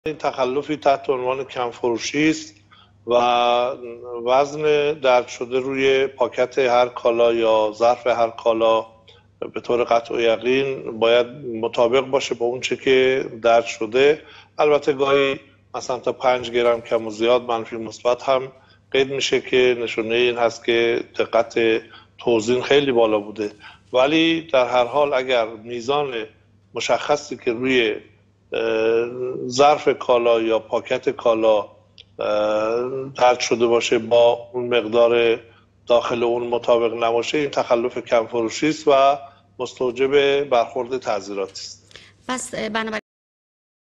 وزن کالاها با وزن درج شده روی کالای فروخته شده همخوانی نداشته باشد تخلف است و برخورد جدی در پی دارد
# تخلف#خبرنگار#نوجوان#